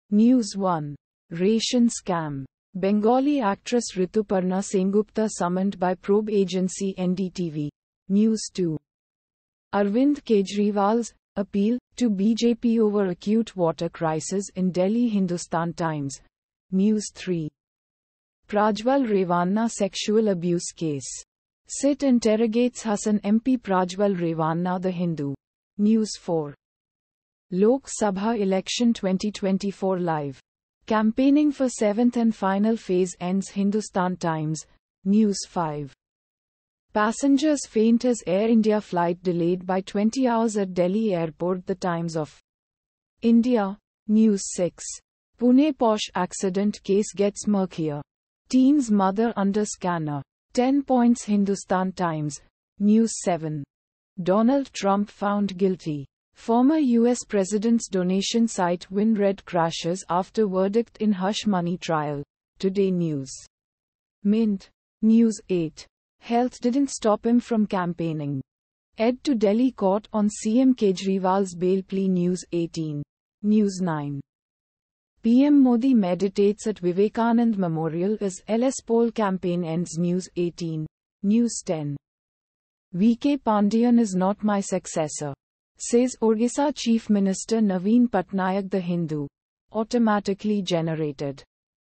News Headlines (India)- 01/06/2024,11:51 AM GMT+5:30
News 1:Ration Scam: Bengali Actress Rituparna Sengupta Summoned By Probe Agency - NDTV
News 2:Arvind Kejriwal's ‘appeal’ to BJP over acute water crisis in Delhi - Hindustan Times
News 3:Prajwal Revanna Sexual abuse case: SIT interrogates Hassan MP Prajwal Revanna - The Hindu
News 4:Lok Sabha Election 2024 LIVE: Campaigning for 7th and final phase ends - Hindustan Times
News 5:Passengers faint as Air India flight delayed by 20 hours at Delhi airport - The Times of India
News 6:Pune Porsche accident case gets murkier, teen's mother under scanner: 10 points - Hindustan Times
News 7:Donald Trump found guilty: Former US president's donation site WinRed crashes after verdict in hush money trial | Today News - Mint
News 8:'Health Didn't Stop Him from Campaigning': ED to Delhi Court on CM Kejriwal's Bail Plea - News18
News 9:PM Modi Meditates At Vivekananda Memorial As LS Poll Campaign Ends - News18
News 10:V.K. Pandian is not my successor, says Odisha Chief Minister Naveen Patnaik - The Hindu

Automatically Generated...